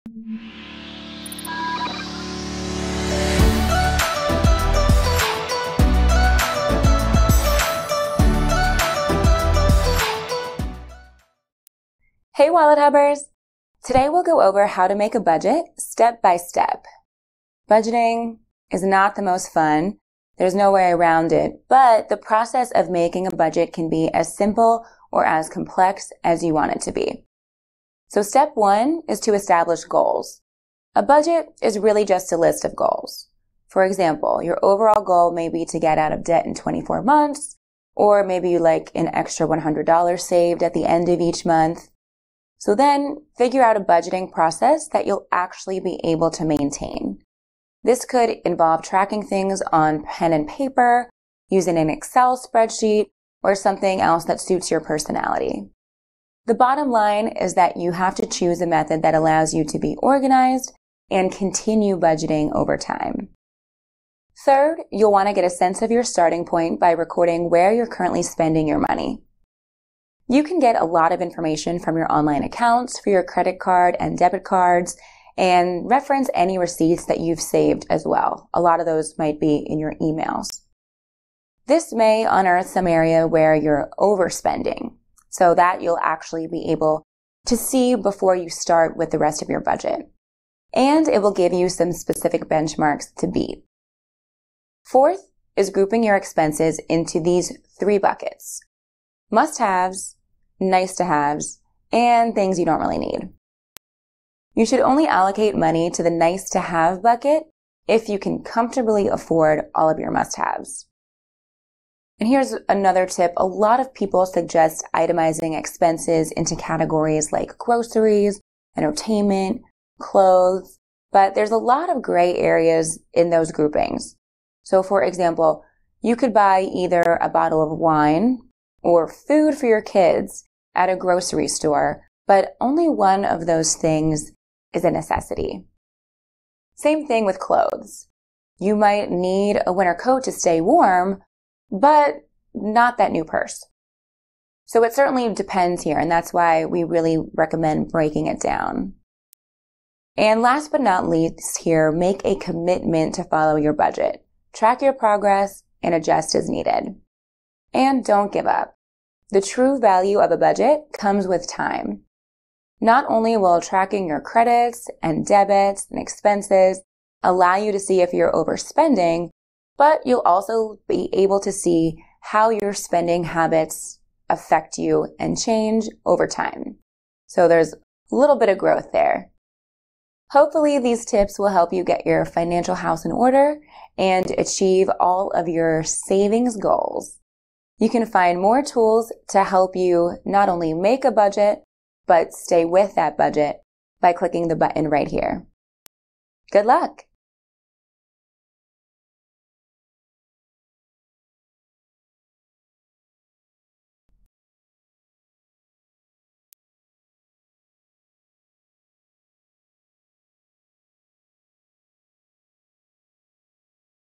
How to Make a Budget
WalletHub's experts explain how to make a budget.